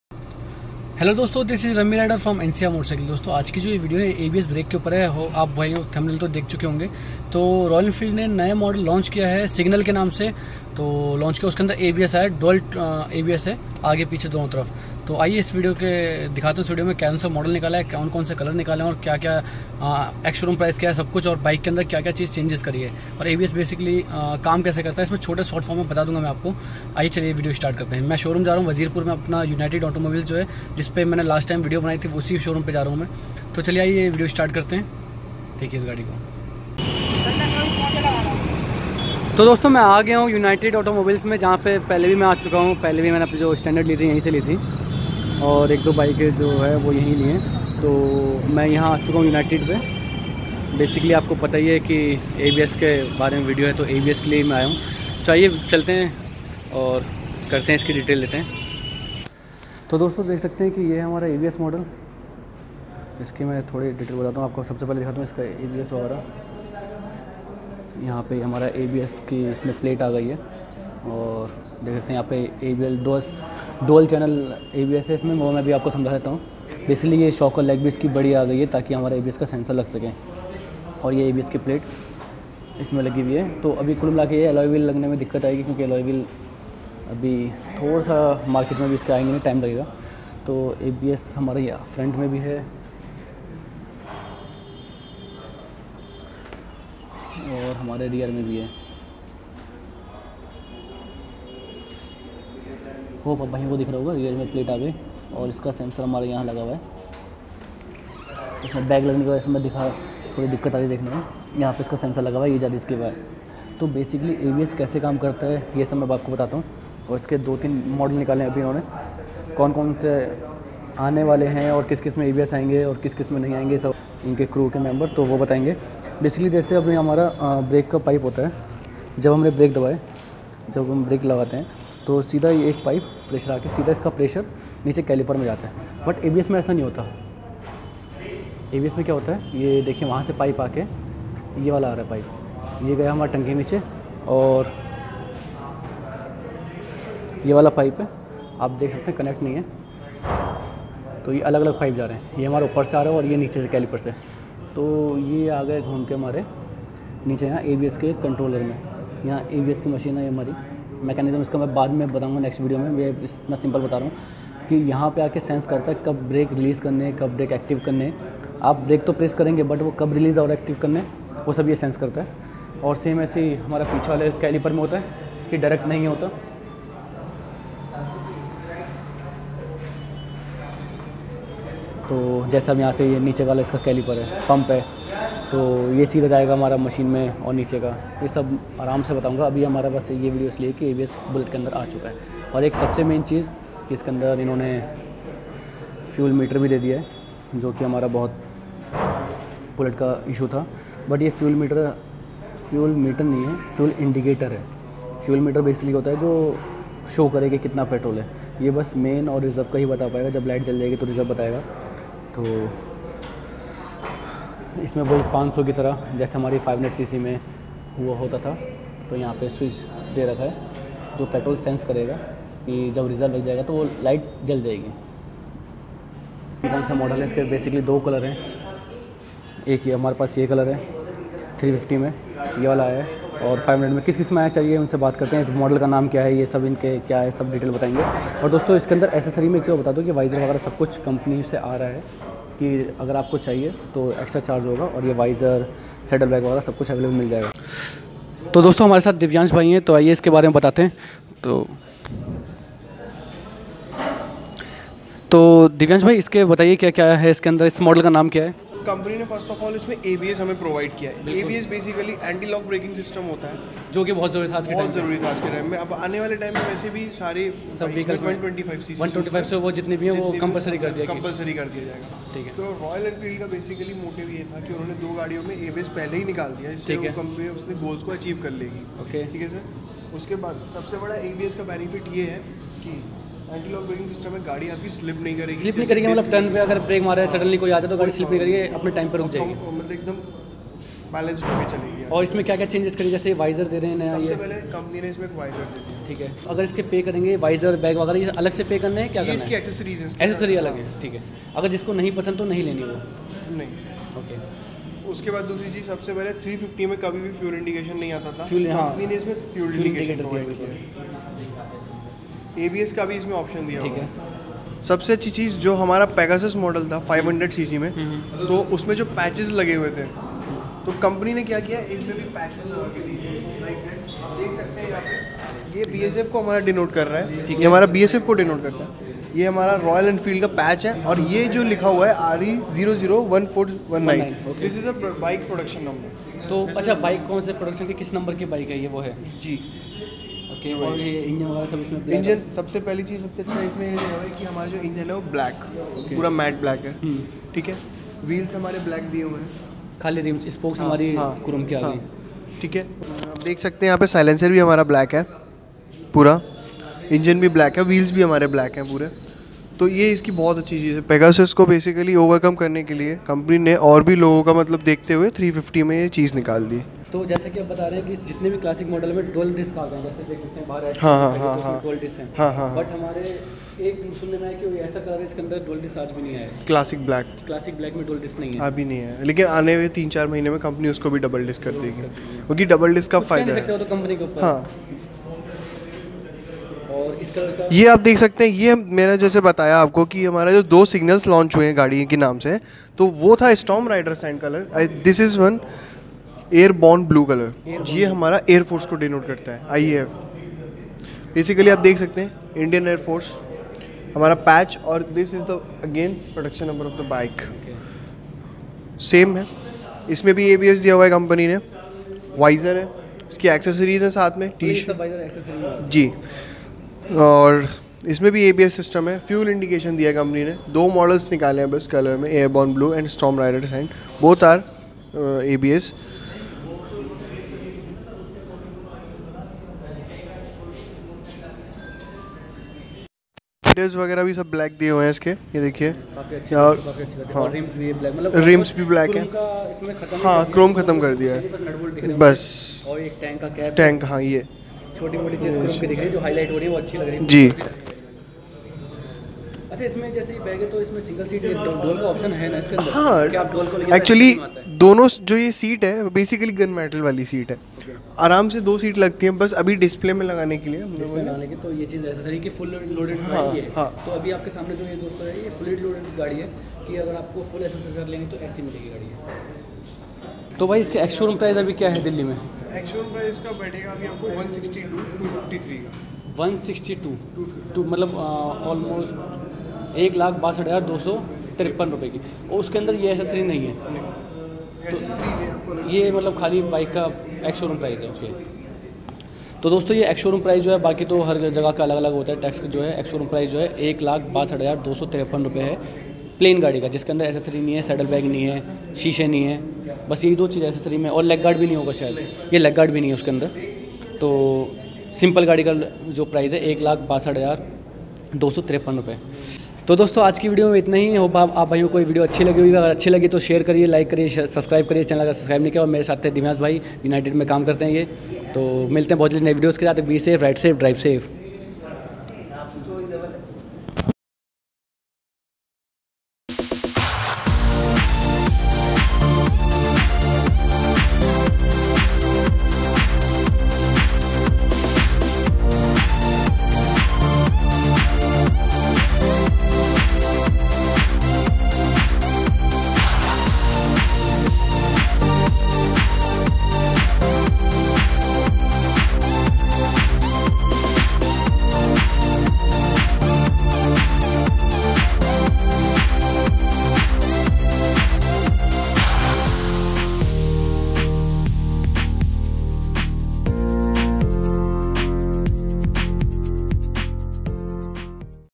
Royal enfield with ABS | signal | ncr motorcycles
Royal Enfield launched a new model bike with ABS system
Name is Royal Enfield  SIGNAL